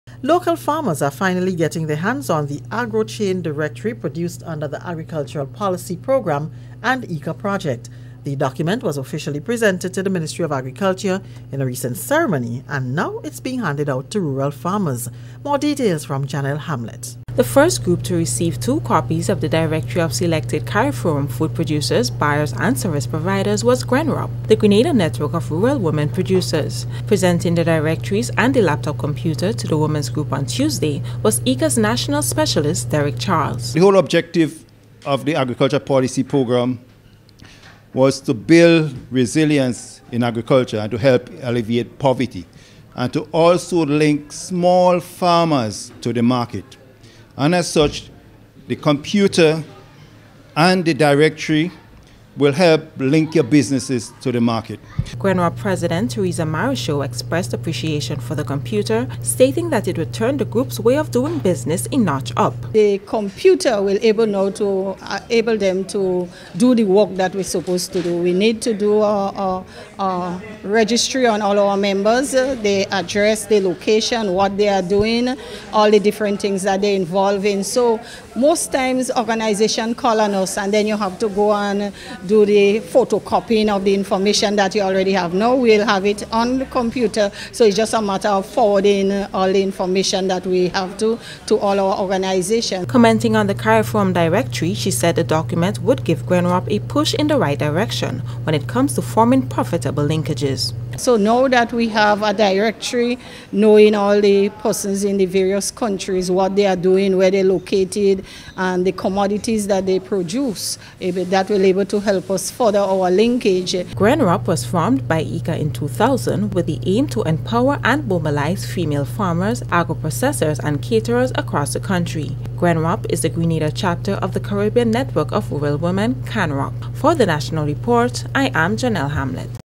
Agro-chain directory now in hands of local farmers
Handover laptop and directory of producers to GRENROP, action of the Agriculture Policy Programme (APP) a project funded under the 10th European Development Fund (EDF) and executed by the European Union (EU) and the Inter-American Institute for Cooperation on Agriculture (IICA).